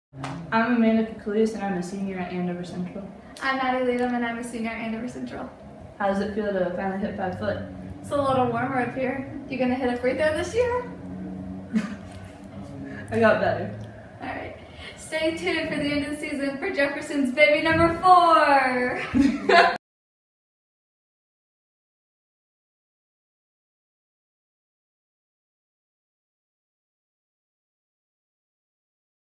2015 Andover Central girls basketball
Andover Central girls basketball players were part of the Wichita Eagle/VarsityKansas.com photo and video shoot with Joanna Chadwick. (2015)